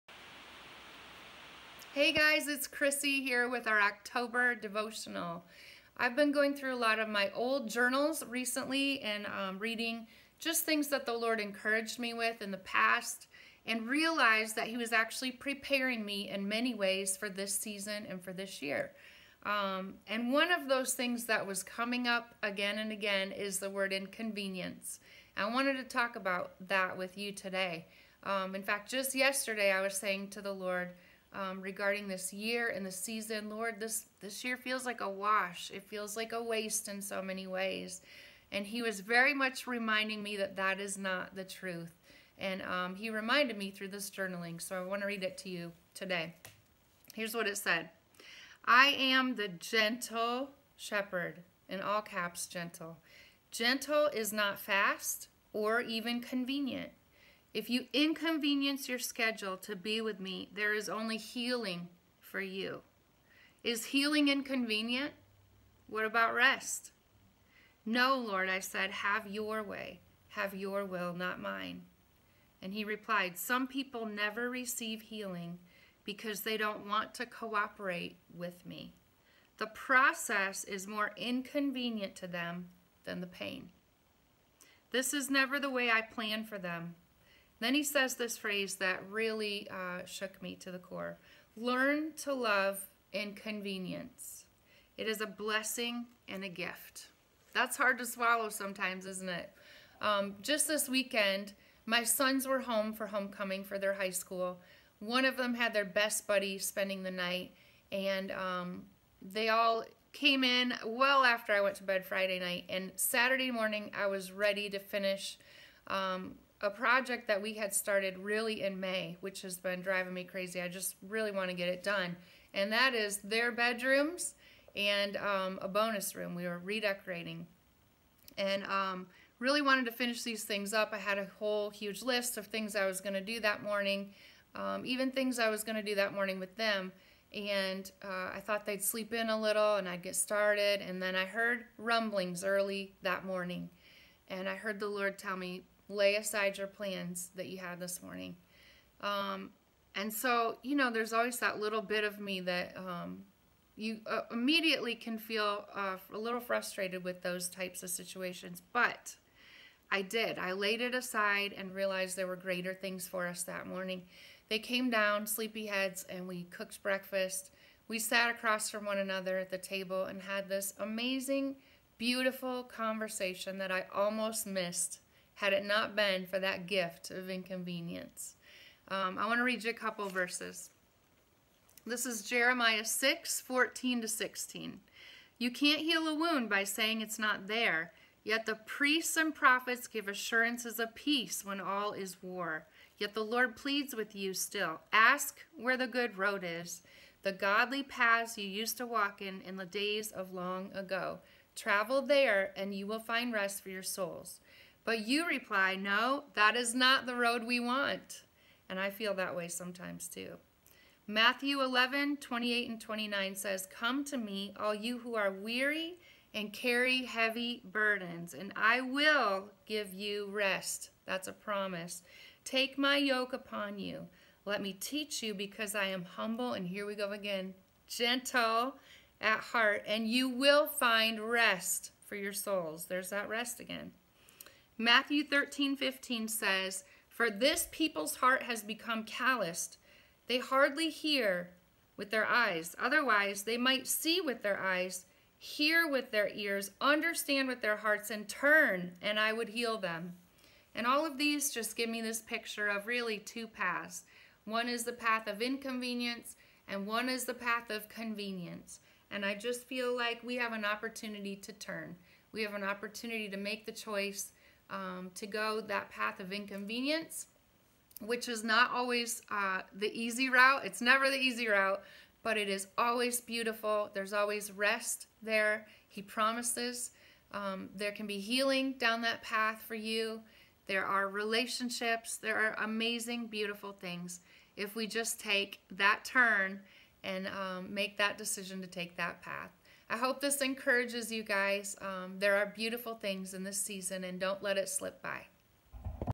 October 2020 Devotional
Learn to love inconvenience.  It's a blessing and a gift.
A devotional from Krissy Nordhoff for the communities.